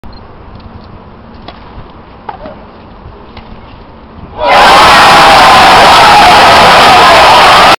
French Open 2015 Final Wawrinka vs Djokovic Match Point
Sorry I was too excited and I stopped filming the video too early before the handshake to celebrate.